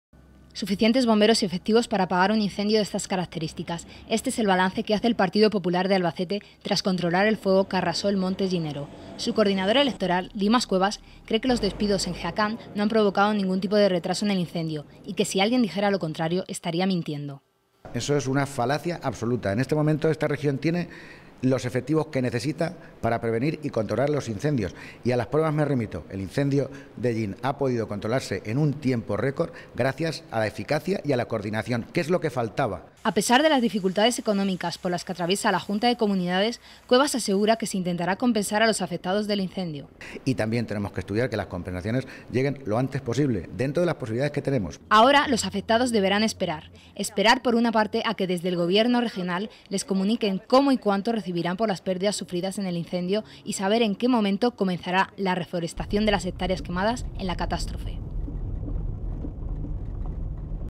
Dimas Cuevas rechaza que haya habido una falta de equipos de extinción en Hellín
El Incendio de Hellín se controló en un tiempo récord. Al menos así lo asegura Dimas Cuevas tras la reunión que ha celebrado la nueva ejecutiva del Partido Popular en Albacete. Cuevas asegura que los efectivos que controlaron el incendio en Hellín eran los necesarios saliendo así al paso de las críticas por parte de Comisiones Obreras que aseguraban lo contrario.